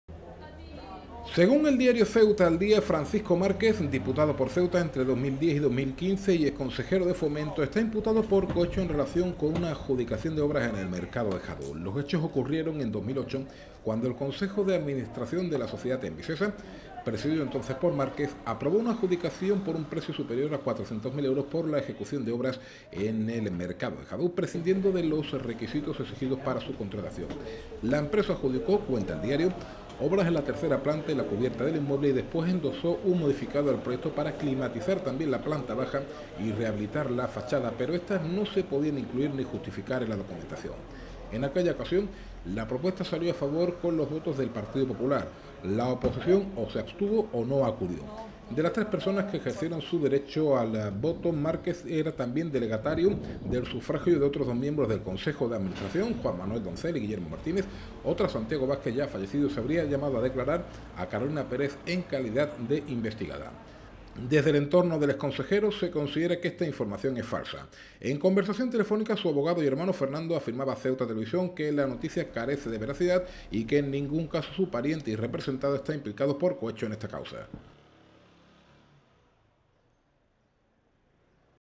La defensa de Márquez niega su imputación por cohecho
Un nuevo frente judicial se abre en torno a la clase política de nuestra ciudad, según informa el diario Ceuta al Día. Se trata, según el diario, de una imputación por cohecho al que fuera diputado por Ceuta en relación a unas obras en el Mercado de Hadú adjudicadas cuando era responsable de EMVICESA. Extremo este negado por el entorno del ex consejero